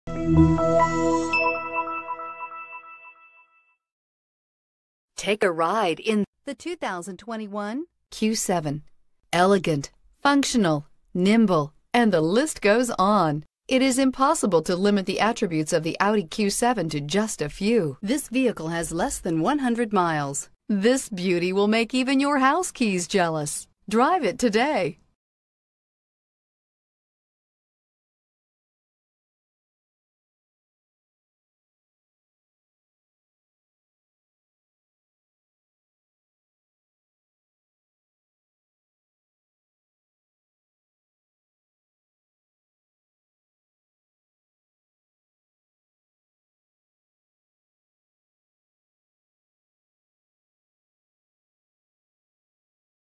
2021 Audi Q7 San Antonio TX 0D014321
2021 Audi Q7 3.0T quattro Premium Plus

Audi Dominion
 21105 IH 10

This 2021 Audi Q7 4dr Premium Plus 55 TFSI quattro features a 3.0L V6 Cylinder 6cyl engine. It is equipped with a 8 Speed Automatic transmission. The vehicle is FLORETT SILVER METALLIC with a BLACK interior. It is offered with a full factory warranty.